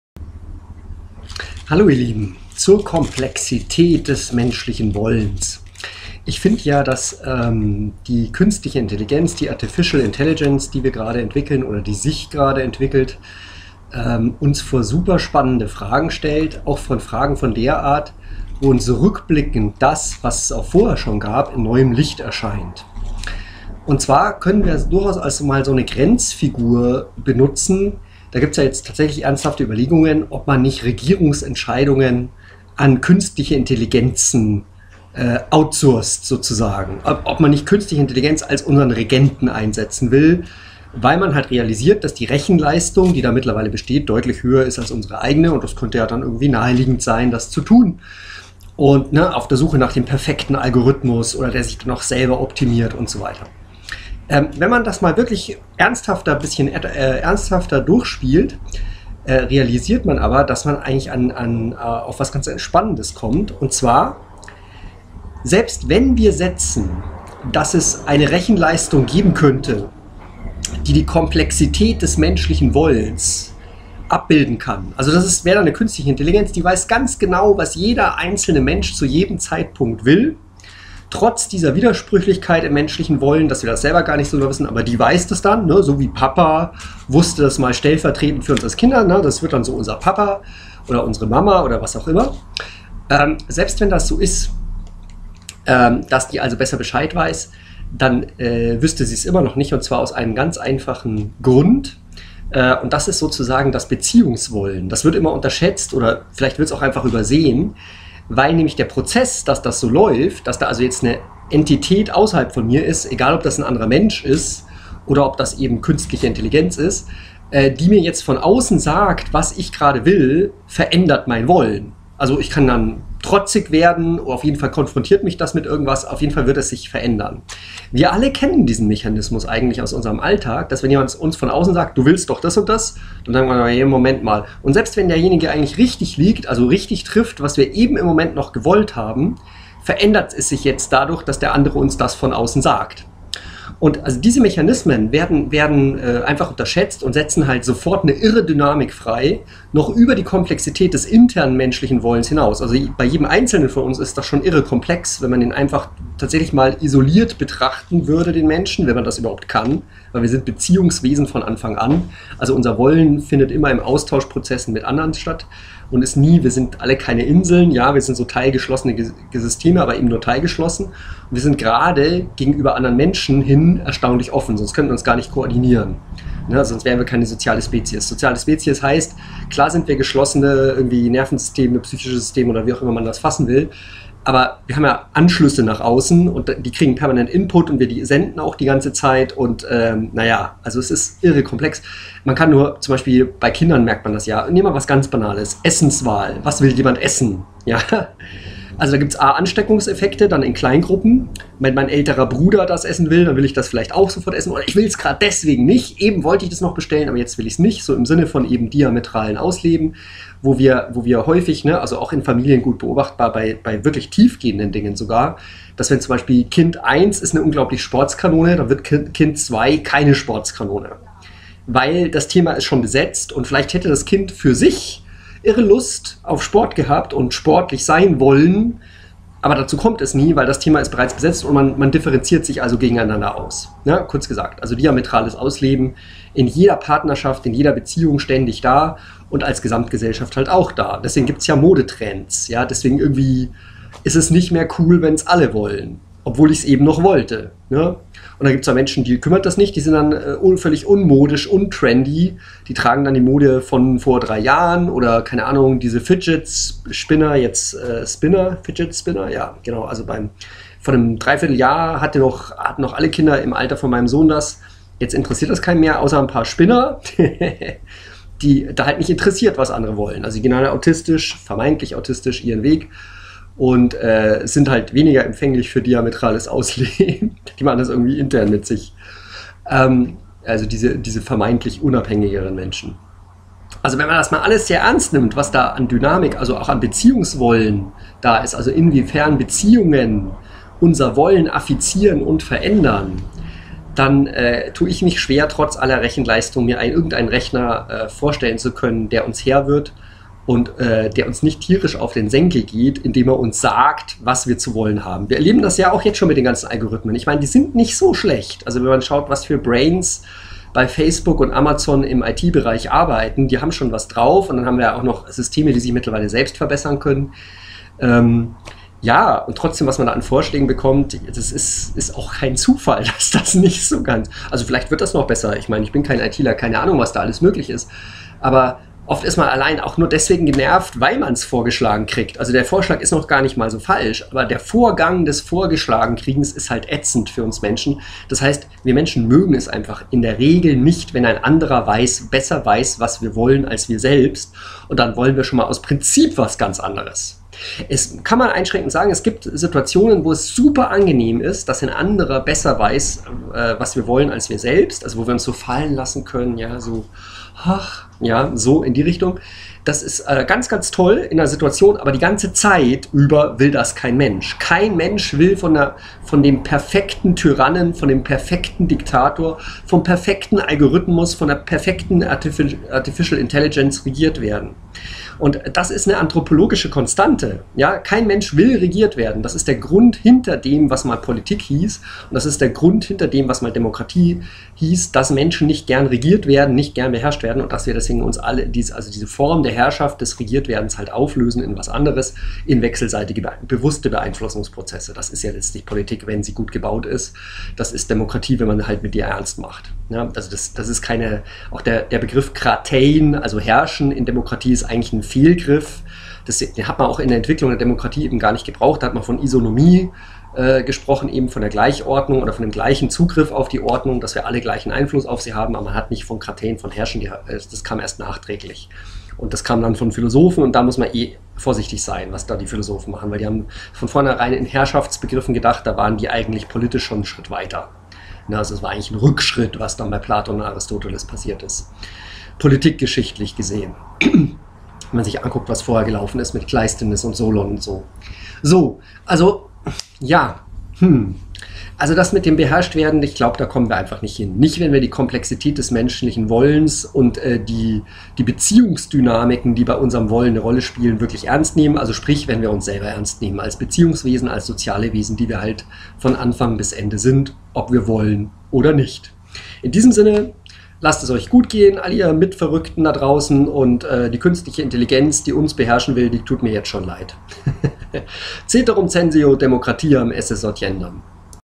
Über die Komplexität des menschlichen Wollens und die Verzweiflung der AI
Viel wird ja spekuliert über Artificial Intelligence und ob wir vielleicht sogar politische Entscheidungen an sie abdelegieren und outsourcen sollen. Einfach, weil ihre Rechenleistung deutlich größer ist als unsere eigene...

Was davon mit Blick auf die Komplexität des menschlichen Wollens zu halten ist, darum geht's in diesem Video.

Über die Probleme, die eine "Expertokratie" systematisch generiert, selbst dann, wenn es keine menschliche Expertise ist, die da regieren soll

Zu Bakunins Theorem, das eine AI-bestückte Regierung ganz genauso trifft, wie eine menschliche Regierung

Dann etwas nochmal deutlich unseriöser Gemeintes - Von Künstlichen Intelligenzen, die GERN beherrscht werden wollen

Warum es schlau ist, wenn wir AI Bürgerrechte und Bürgerpflichten einräumen - kurz: wenn wir sie wie Mitbürger behandeln